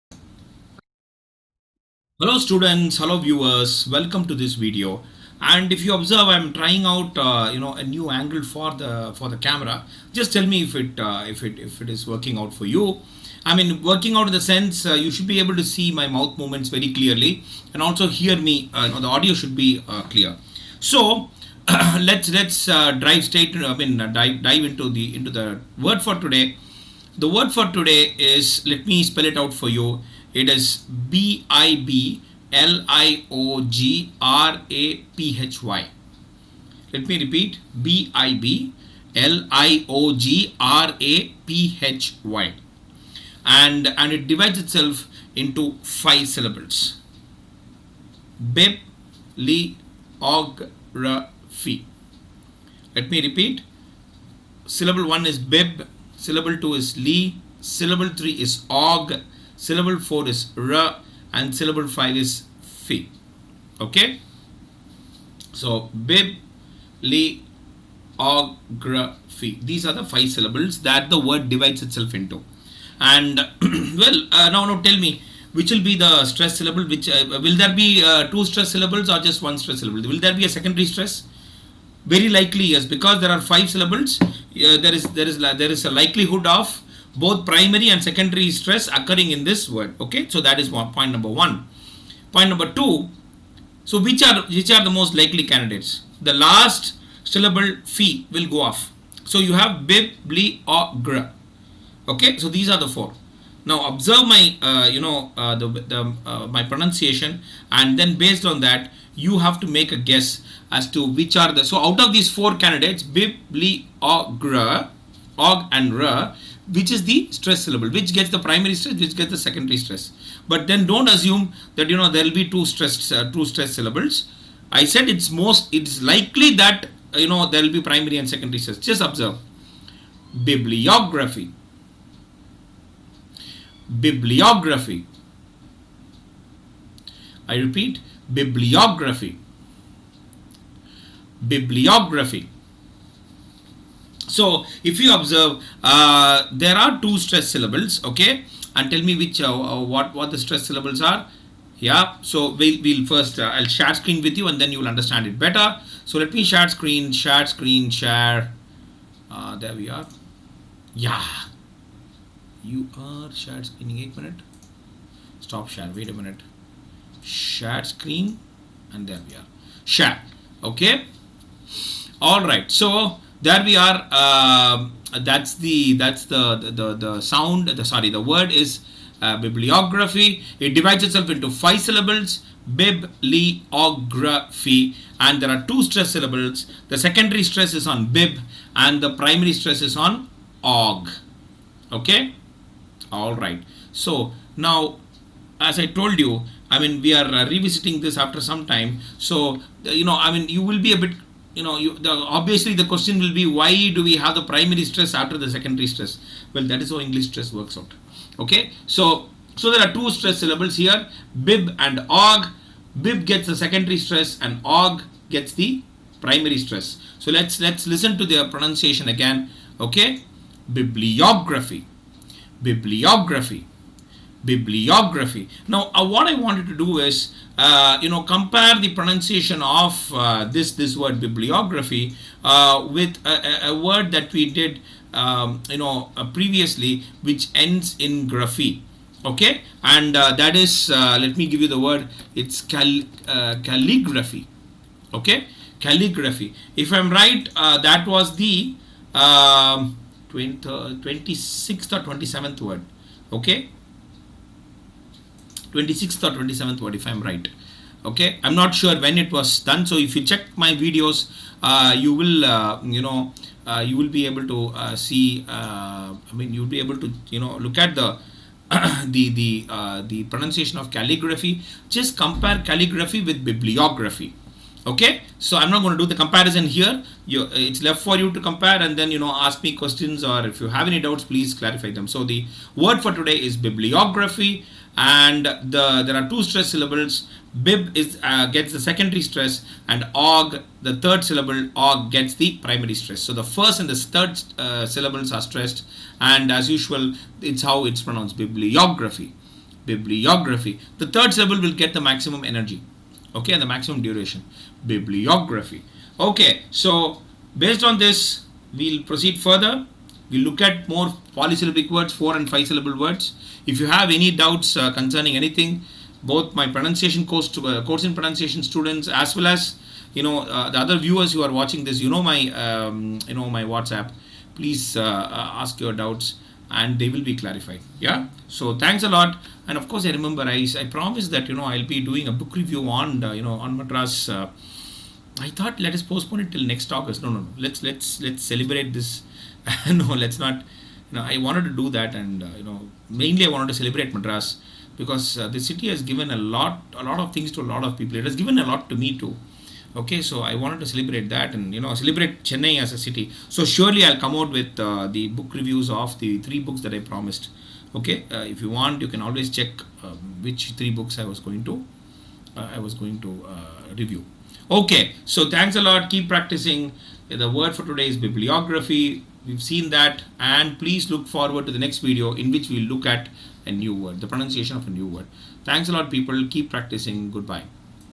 Bibliography Pronunciation Prof RBS
Dear Students, Dear Viewers. Today we look at the pronunciation of the word 'bibliography'. This word divides itself into five syllables and has a primary- and a secondary stressed syllable.

Regards
Prof.R.Balasubramanian